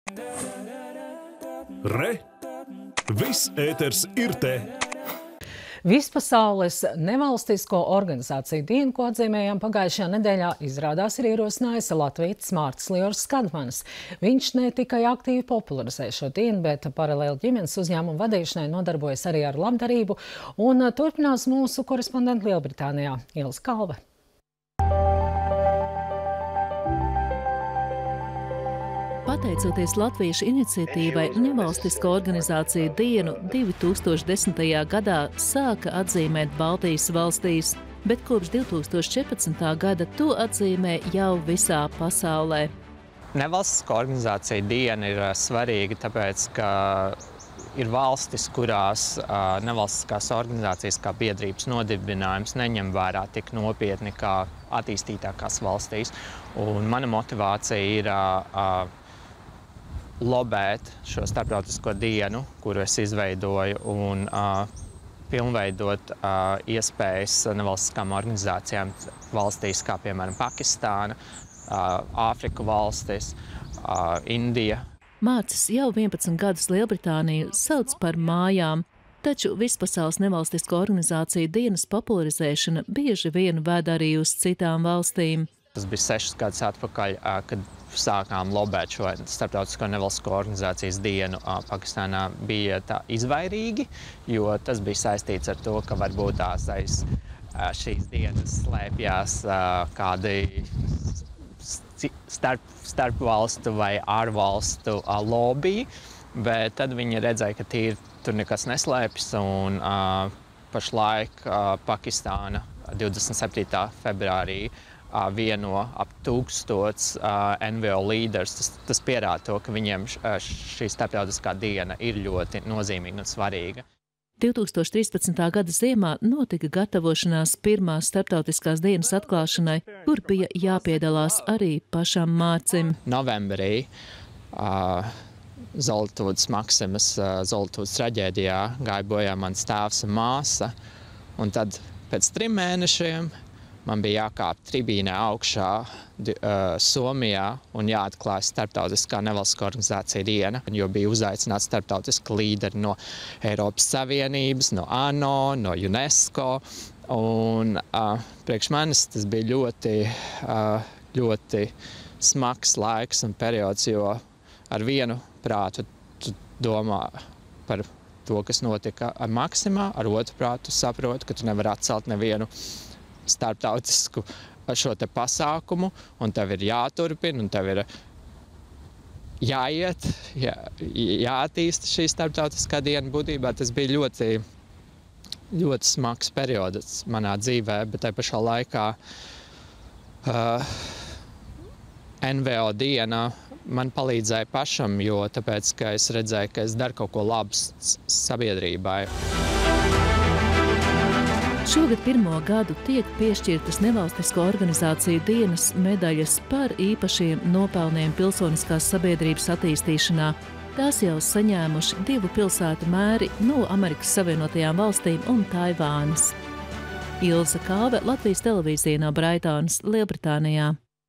Latvietis popularizē NVO dienu
Vispasaules Nevalstisko organizāciju dienu, ko atzīmējām pagājušajā  nedēļā, izrādās, ir ierosinājis latvietis Mārcis Liors Skadmanis. Viņš ne tikai aktīvi popularizē šo dienu, bet paralēli ģimenes uzņēmuma vadīšanai nodarbojas arī ar labdarību. Turpina mūsu korespondente Lielbritānijā Ilze Kalve.